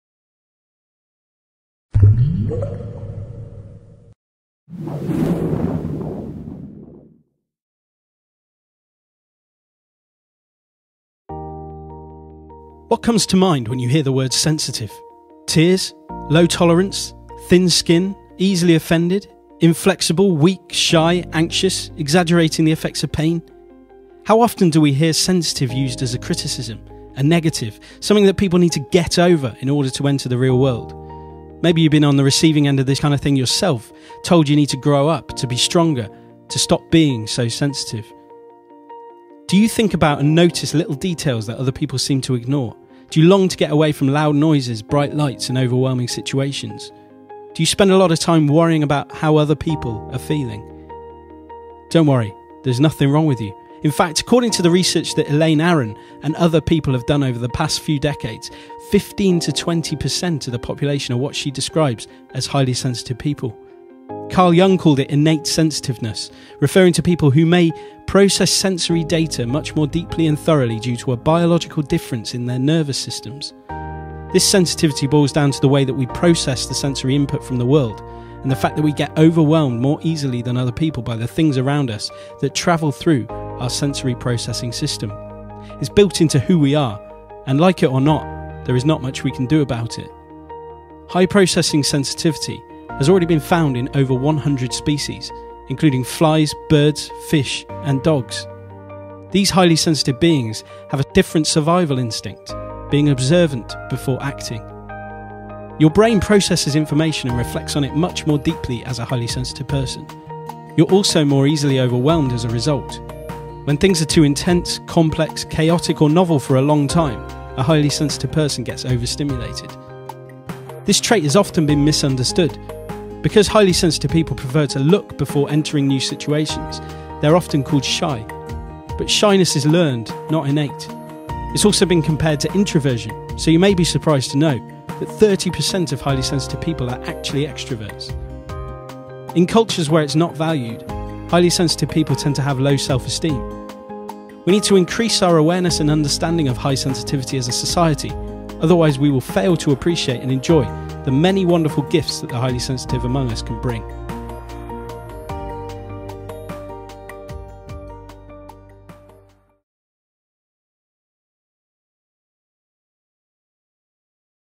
Are You a Highly Sensitive Person?
Chances are you've heard terms like "highly sensitive person", "environmental sensitivity", or "high sensory intelligence" in recent years. They are different descriptions for the same underlying trait known as sensory processing sensitivity (SPS).

But what does that actually mean?